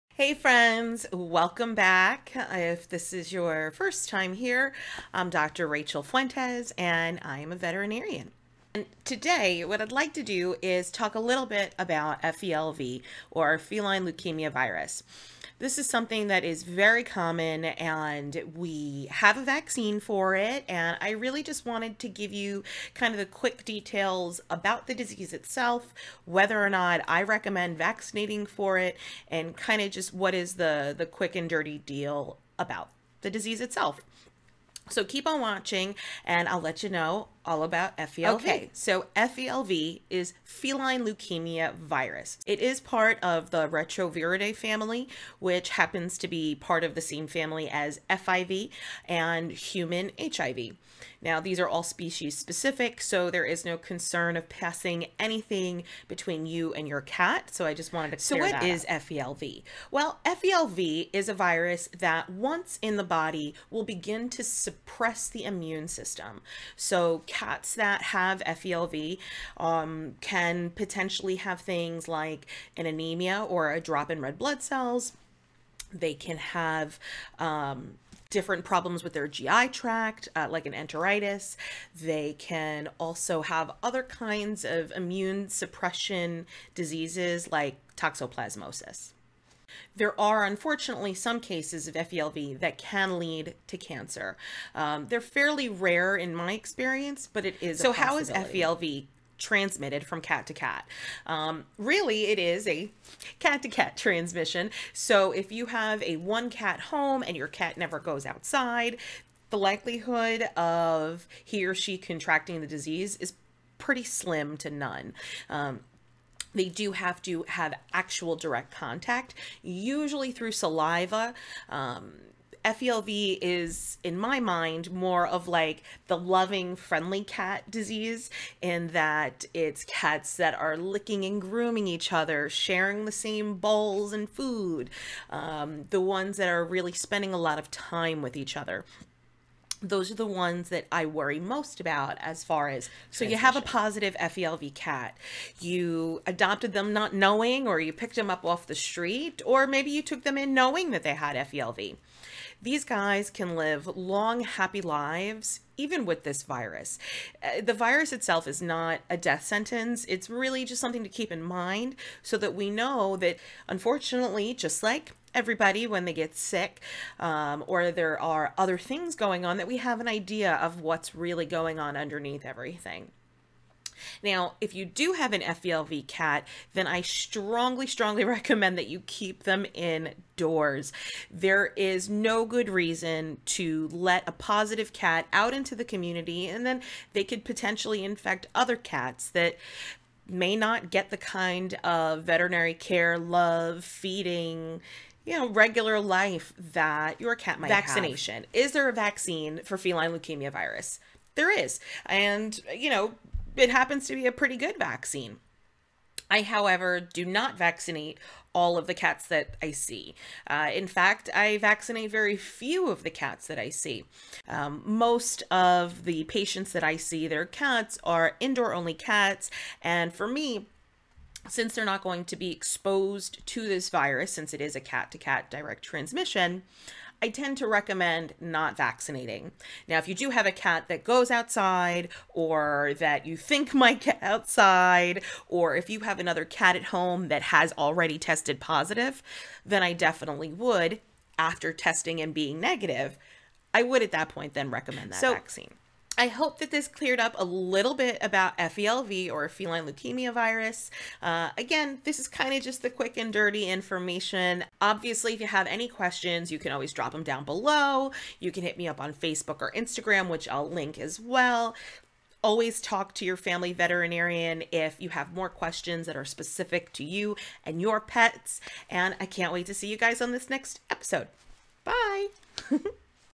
The 5 minute rundown: Feline Leukemia Virus (FeLV)
Hi friends,  this video is a 5 minute quick explanation on Feline Leukemia Virus also known as FeLV.  I touch on: Who gets FeLV, what are the possible clinical signs, whether or not you have to euthanize an FeLV+ cat, and if I recommend vaccinating for it.